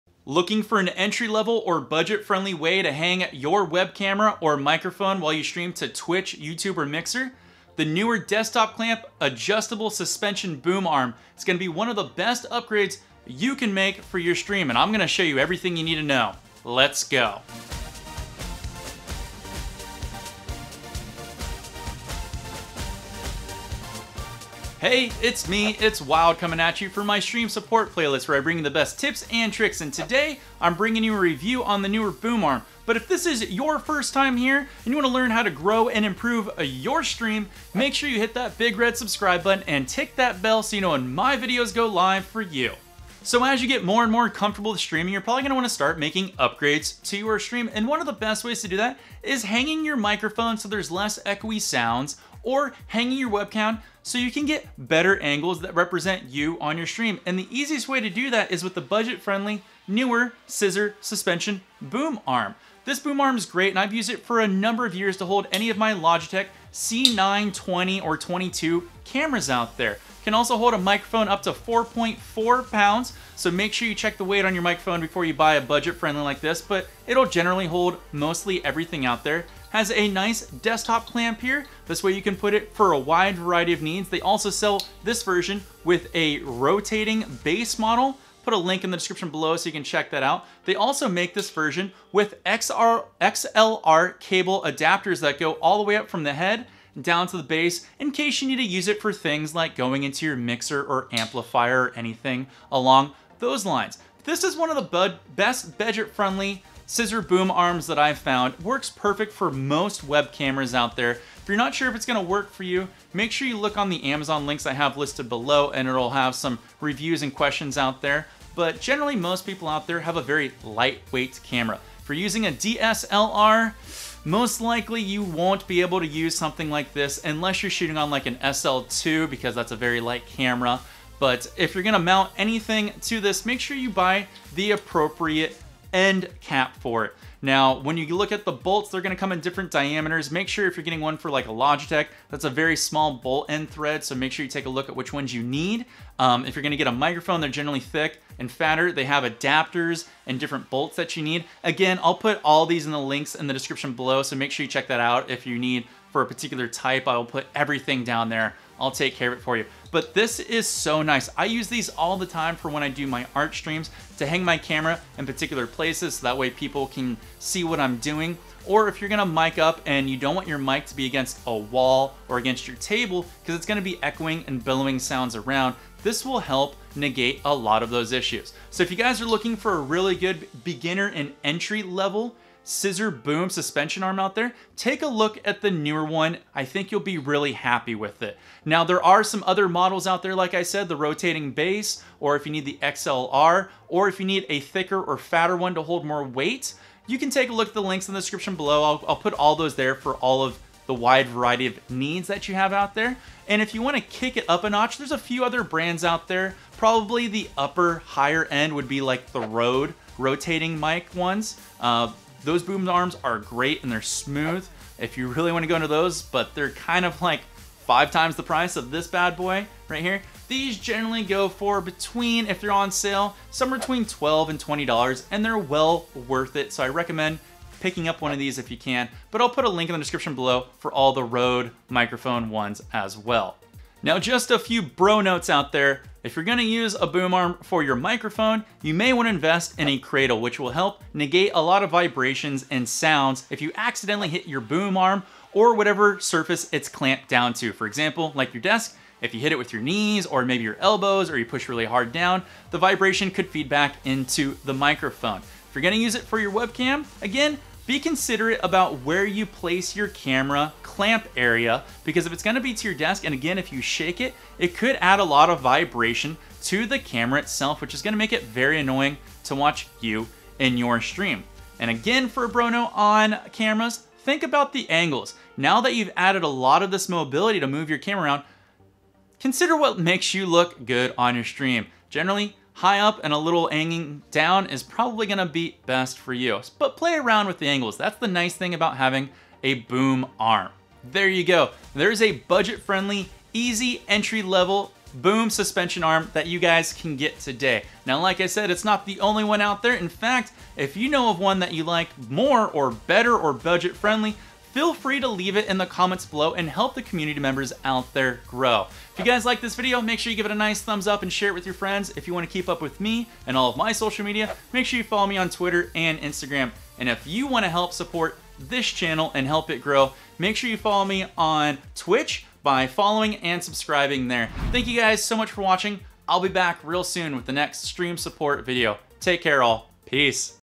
Best Budget Boom Arm For Gamers & Streamers
Looking for the best budget friendly way to hang your mic or camera the Neewer Adjustable Microphone Suspension Boom Scissor is for you!

If you are looking to step up your game for your streaming rig. The NEEWER Adjustable Microphone Suspension Boom Scissor Arm is a great tool to add mobility to your microphone and camera while also freeing up desk space. The Neewer suspension boom arm is the best bang for your buck, perfect for the entry level streamer or pod caster. This video is perfect for any Twitch, YouTube or Mixer streamer looking to grow and refine there stream channel.

Helpful Links:

🔷 Neewer Suspension Boom Arm for Web Cam
🔷 Neewer Suspension Boom Arm for Mic
🔷 Tonor T20 Boom Arm
🔷 Rode PSA1 Boom Arm
🔷 Blue Compass Boom Arm